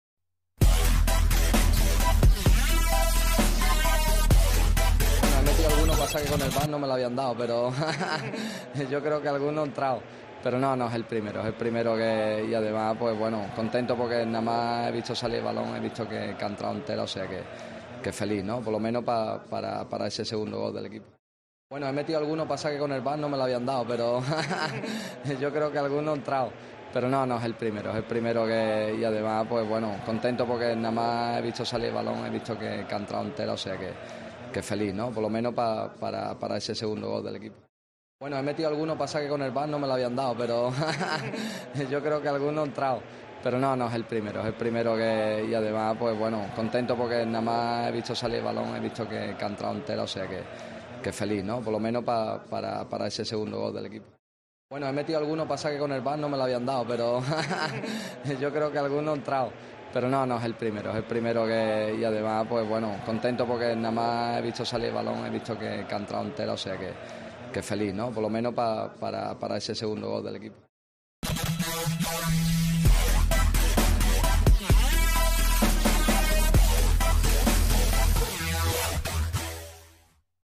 Joaquín habla de su gol olímpico al Valencia en Copa gracias al VAR
Gol olímpico de Joaquín en Copa del Rey gracias al VAR ante el Valencia